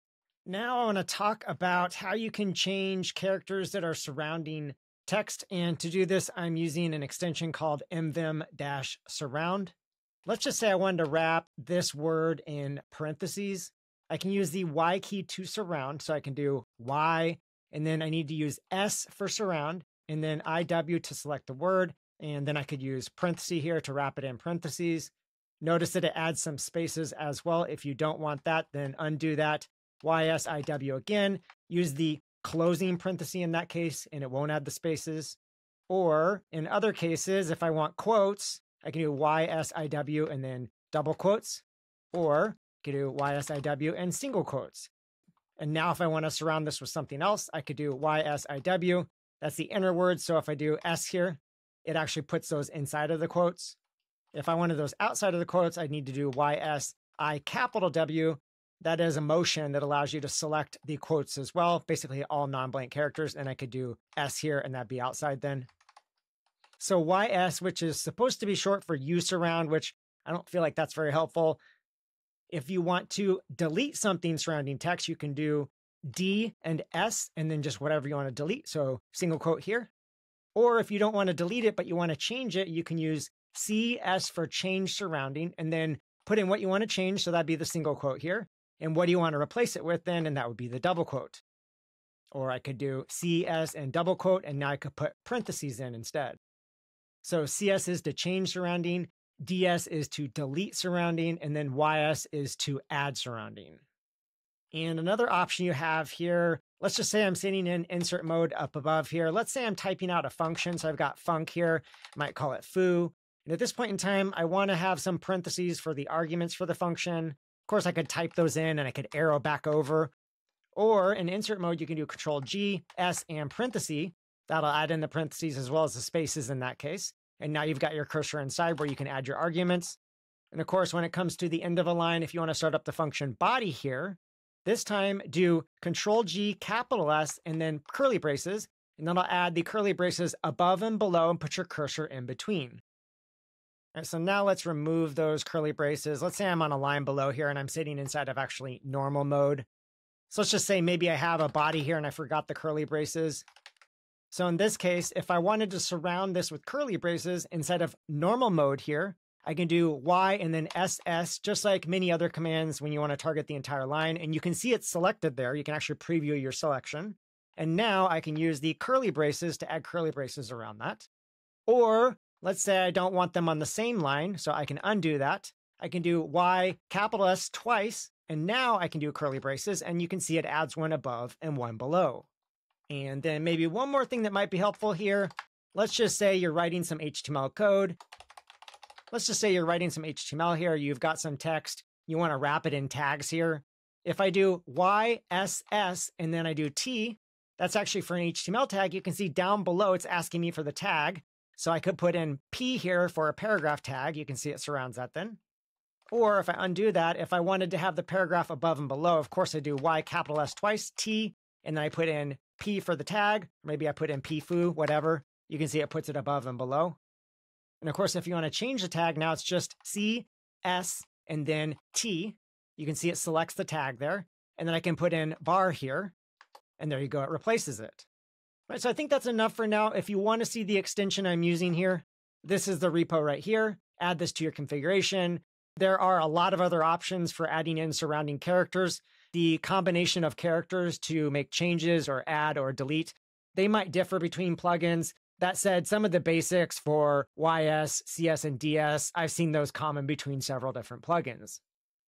Explicitly Wrap Text in {} () "" '' // - Instead of Auto Closing Pairs
Instead of predicting when to insert matching pairs of characters, explicitly request it with a few keystrokes using nvim-surround.